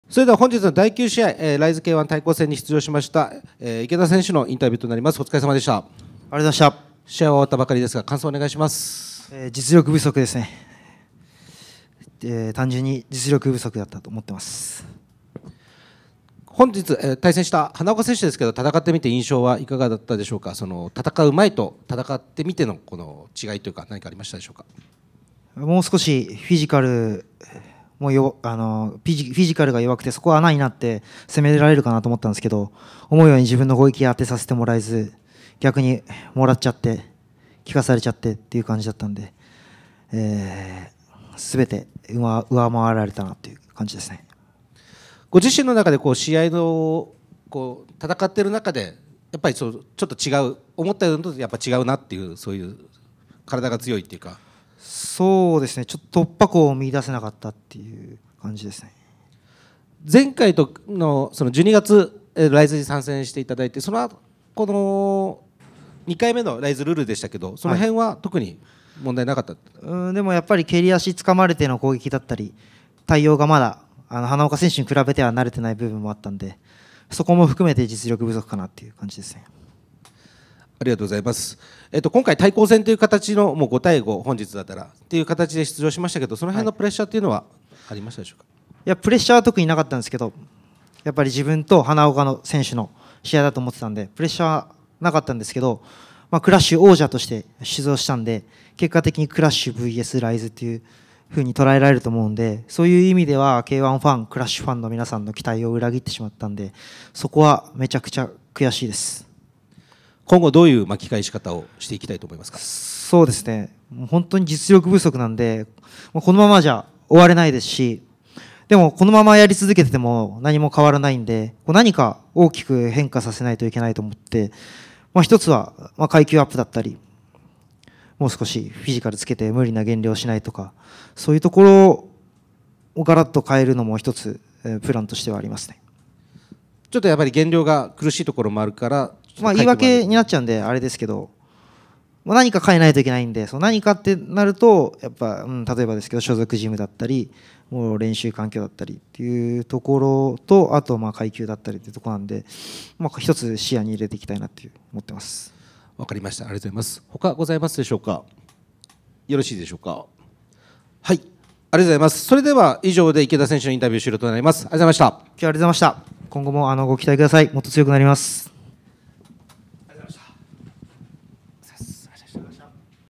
K-1・池田幸司【RISE ELDORADO RISEvsK-1対抗戦】花岡竜に０−３判定負けで「実力不足」と猛省
「RISE ELDORADO」（１７日、東京体育館）
この日の第９試合、RISEとK-1の対抗戦としては３試合目で、K-1代表として出場したKrushバンタム級王者の池田幸司は、RISE代表の花岡竜に０−３判定で敗れた。

　池田は会見場で「実力不足」を痛感したと反省の弁を口にした。